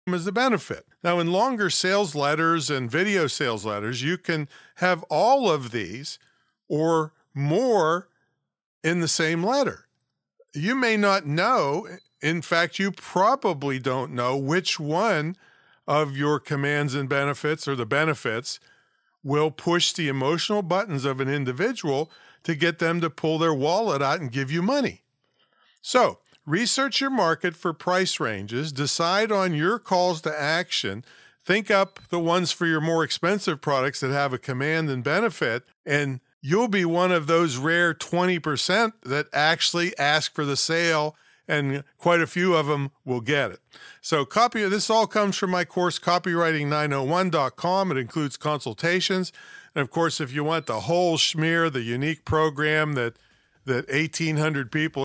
Research your market for price ranges
Today, we're back to our copywriting series with Calls to Action. This is really important if you want to get the money. Now, the beginning of this series started at episode 836.

00:23 Tom's introduction to Calls To Action

01:46 Over 80% of sales people never ask for the sale

03:16 Using calls to action

04:56 Research your market for price ranges